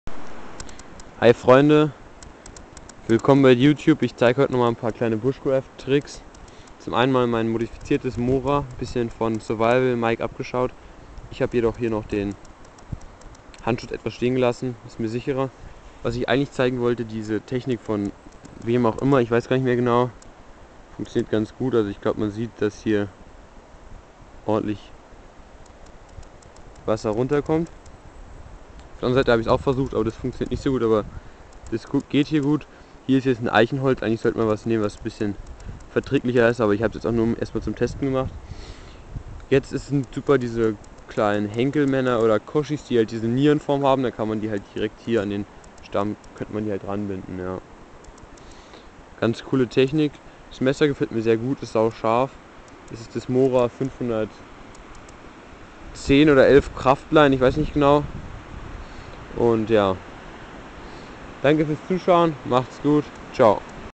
Birkensaftgewinnung und eine neue Messermodifikation
Birkensaftgewinnung und ein neues Messer:)